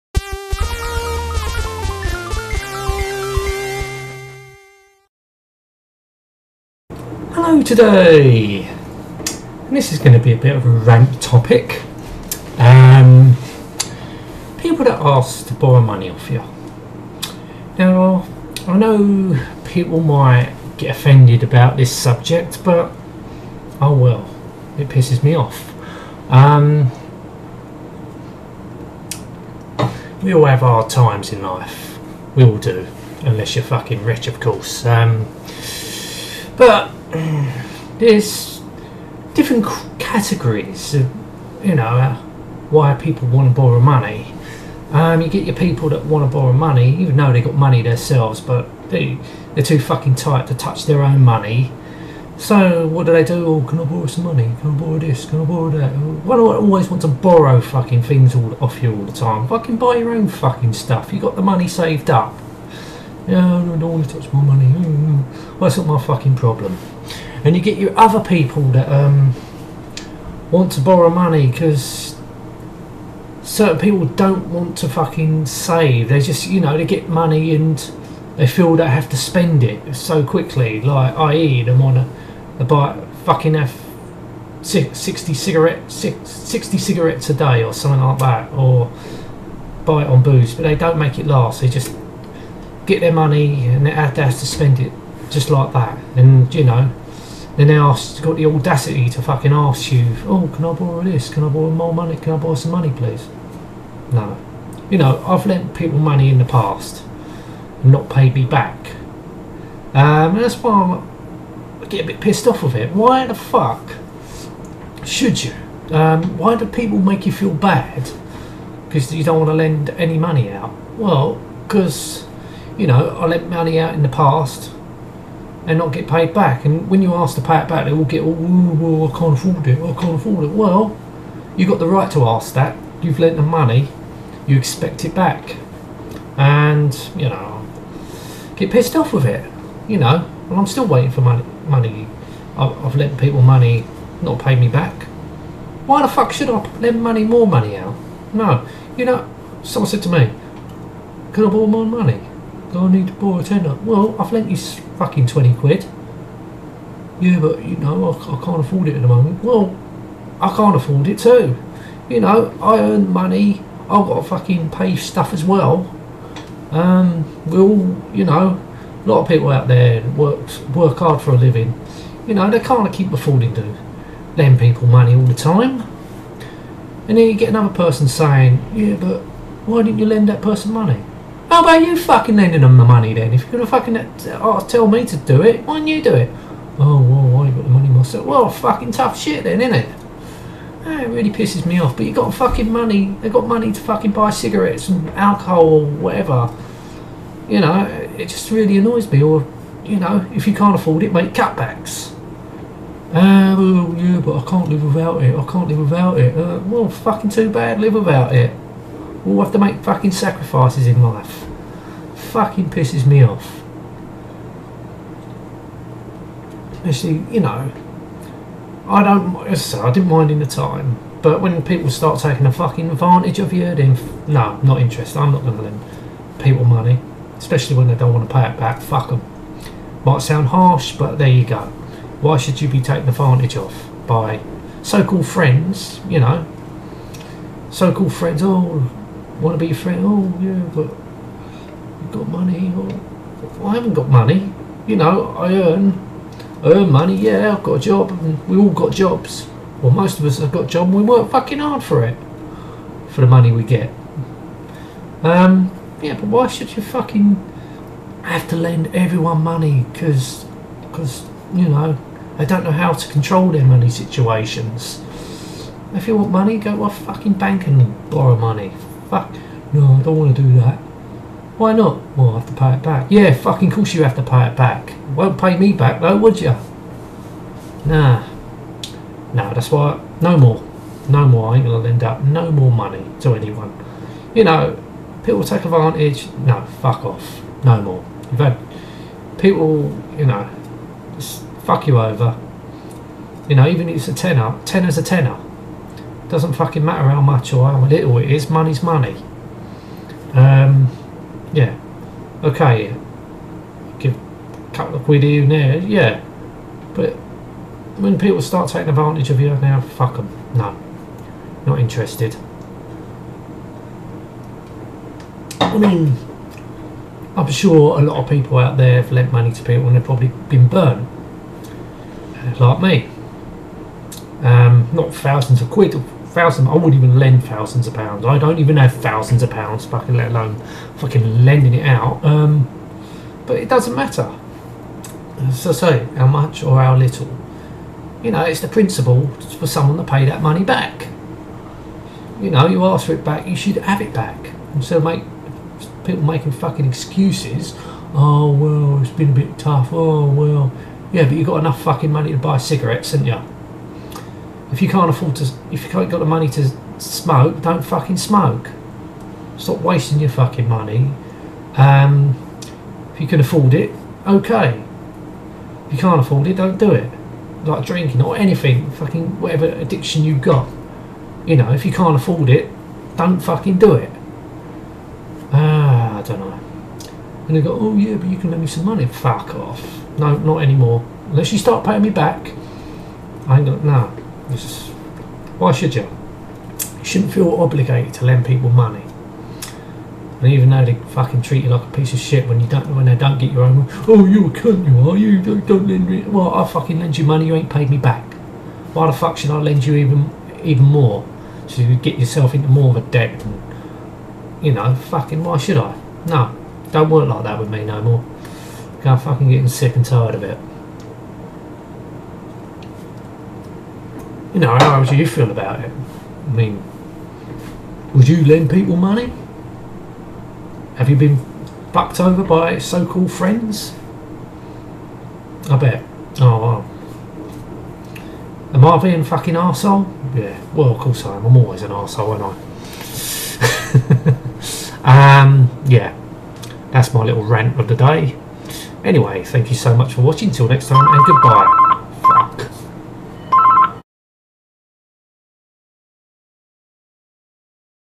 Talking On The Camera 03/09/2016 - People That Ask To Borrow Money From You
Having a rant about people that ask to borrow money from you. Warning! This video has swearing.